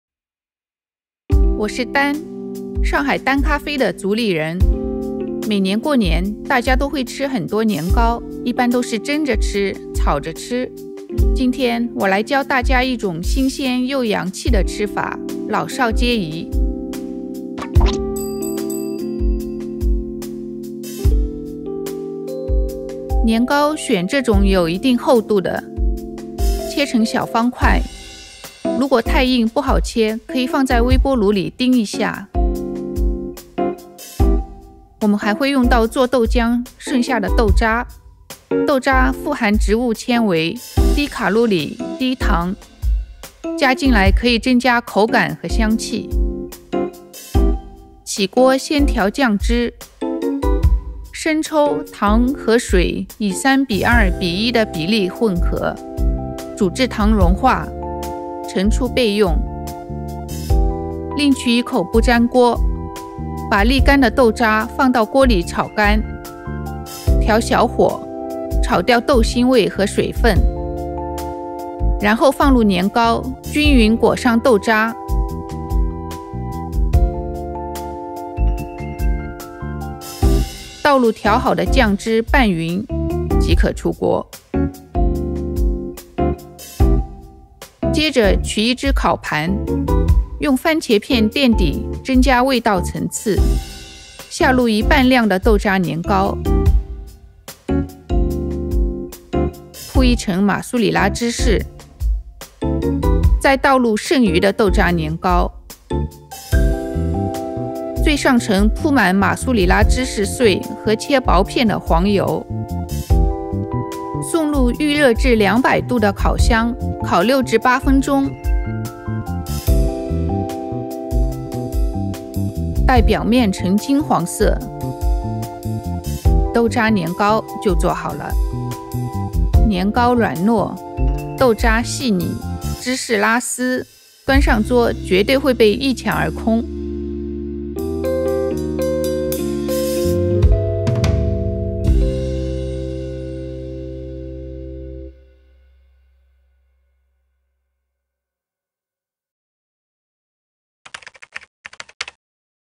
美食台 | 年糕也能拔絲，你一定要試試！
相信每年春節大家年糕都備得很足吧？也吃得有點膩。今天教你一種新鮮洋氣的方法，會先用做豆漿留下來的豆渣炒一下年糕，然後配上番茄、起司烤一下，年糕軟糯、豆渣細膩、起司拉絲，端上桌絕對會被一搶而空！快在鄉親們面前露一手吧！
I believe you all make sure you have plenty supply of rice cakes during the New Year festival. And you may also be tired of eating them the same way all these years. Today we bing you a fresh new rice cake recipe. First stir-fried rice cakes with bean residue left from making soya-bean milk, and then bake with tomatoes and cheese. Creamy rice cakes coupled with delicate bean residue, supplemented with rich cheese, the tray is sure to be emptied before you know it. Come on show the folks what you are capable of.